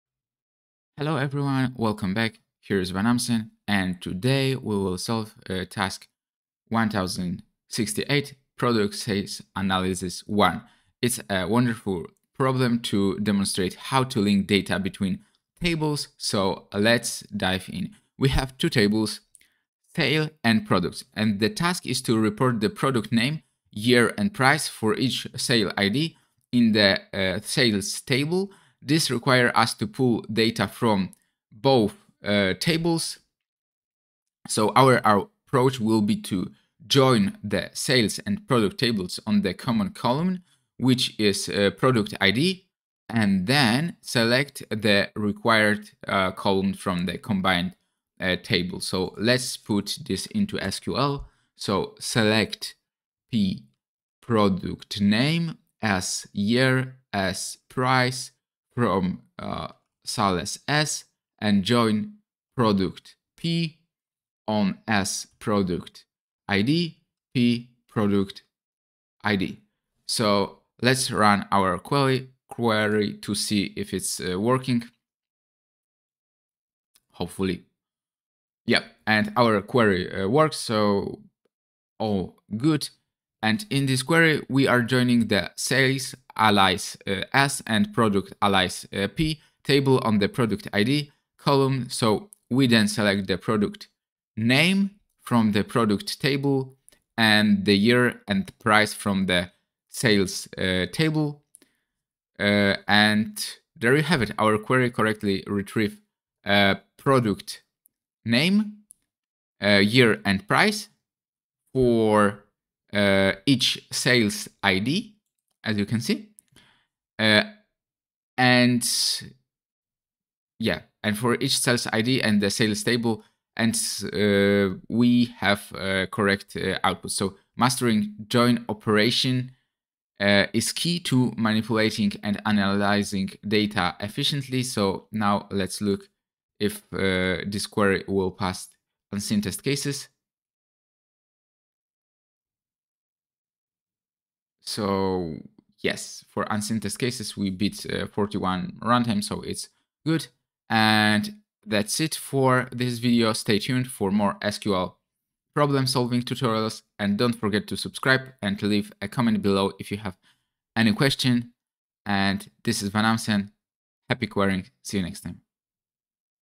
Solving LeetCode SQL Problems: 1068. Product Sales Analysis I - Unraveling Sales Data
In this episode of our LeetCode SQL series, we're going to tackle "1068. Product Sales Analysis I". This problem is a real-world scenario where we need to connect information between two tables, Sales and Product, to perform data analysis. Our objective is to construct an SQL query to report the product_name, year, and price for each sale_id in the Sales table.

Starting with a brief introduction of the problem, we'll walk through the process of crafting the SQL query, explaining the key concepts, such as JOIN operations, and why we use them. The practical insights gained from this tutorial will be useful for solving similar problems and performing complex queries on multiple tables.

Join me, VanAmsen, as we conquer this exciting SQL problem. Leave your thoughts or questions in the comments below. Let's get into some SQL action!